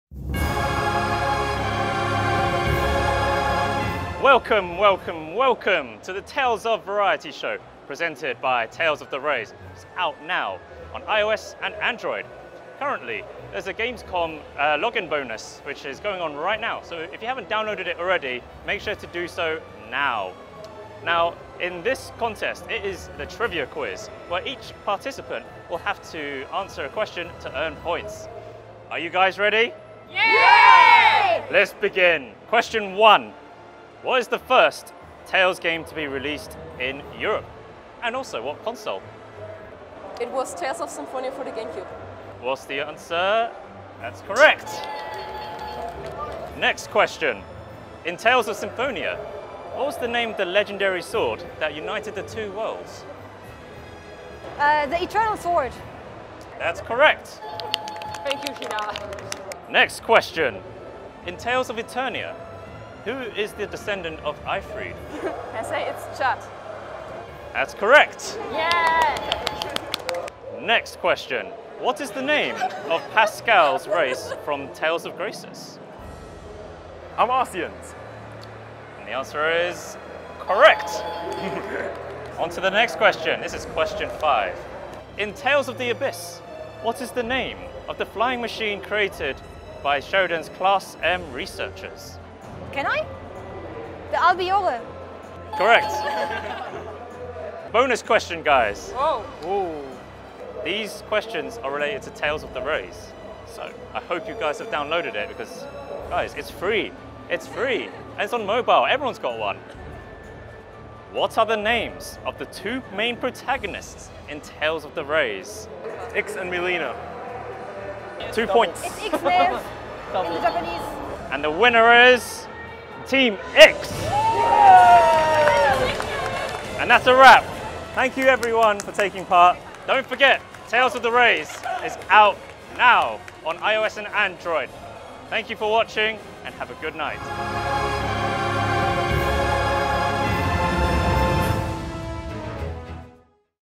Tales of the Rays - Gamescom Trivia Challenge #1 | iOS, Android
Trivia challenge! How much do you know about the Tales Series? We put these fans to the test at Gamescom as we celebrated the release of Tales of the Rays, the ultimate cross over Tales game out now on iOS and Android!